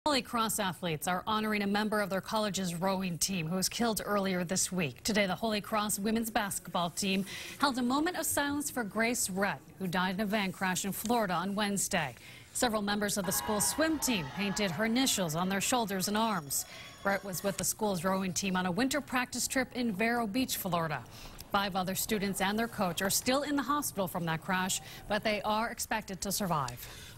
Holy Cross Athletes Honor Grace Rett
WBZ-TV's Katie Brace reports.